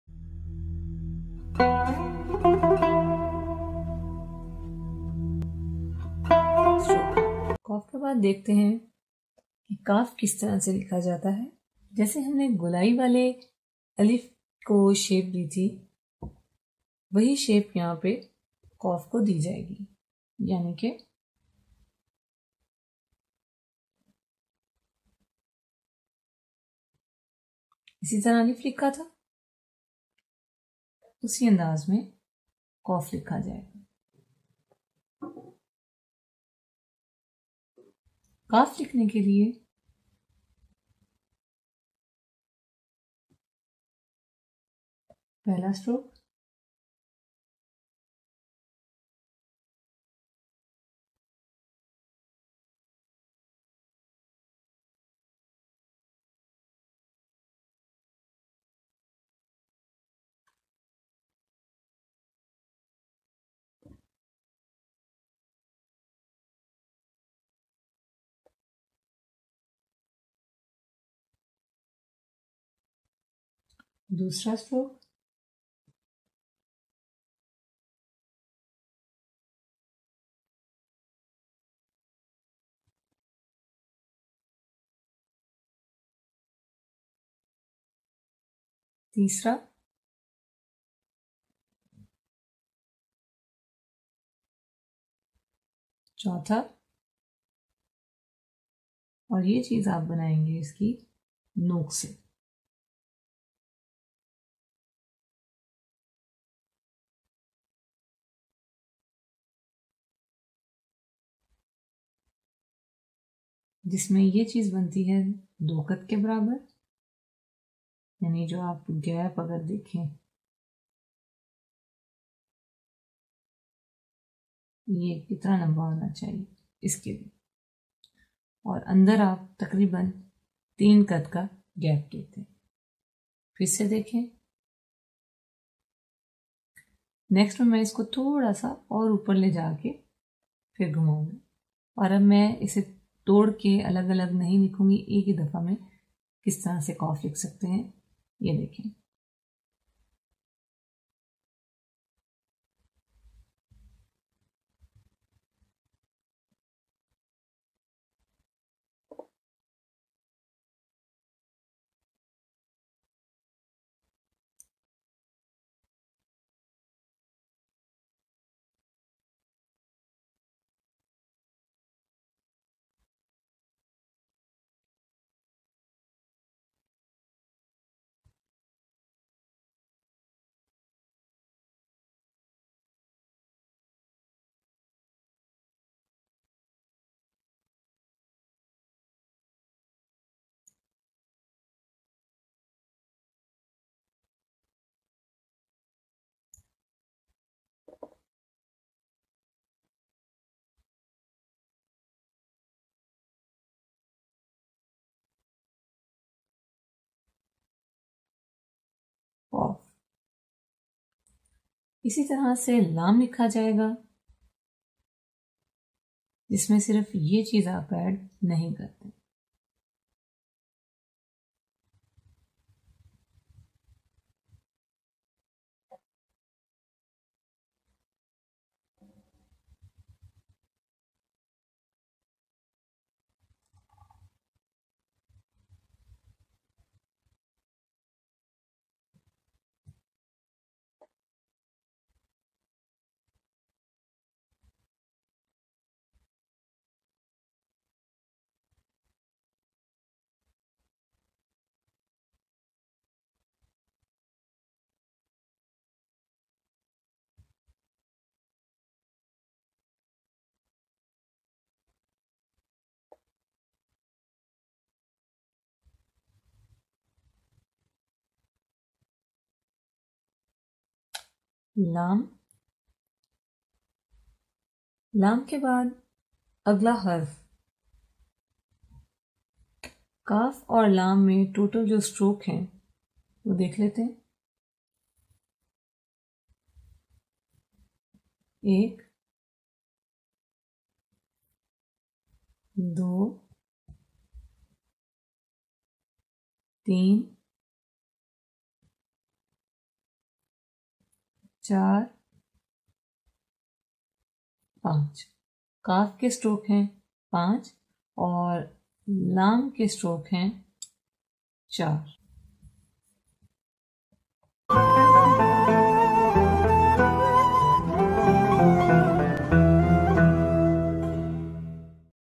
How to Write Kaaf, Laam / Diwani Script with Qalam / Lesson 13 / Arabic Calligraphy
In today's video we will see How to Write Kaaf, Laam / Diwani Script with Qalam / Lesson 13 / Arabic Calligraphy

Now You Can Purchase my Artworks.